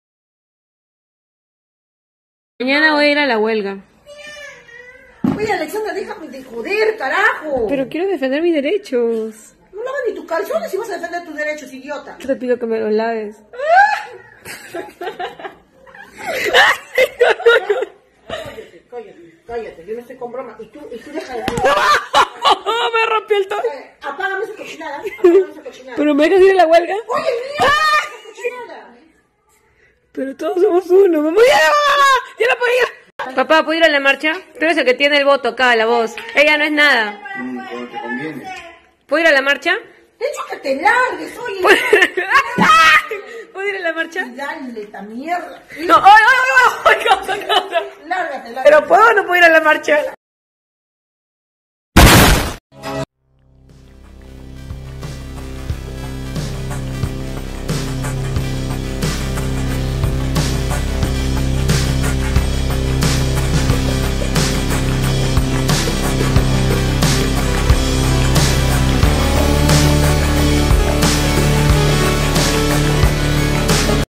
|| 🇵🇪 Puedo ir a la marcha 🇵🇪 ||meme original??《 Lion_Pink YT 》
Este es un día normal con mi familia Ú-Ù👌💕💕💕
Y eso que faltan los duendes de mis hermanos qwp
Cierto cuídense el covid sigue afuera 《asi que usen tapa bocas y la vence las manos con jabón xd》
Bueno ahora si Chaufita mis leoncitos y mis amores Û3Û✌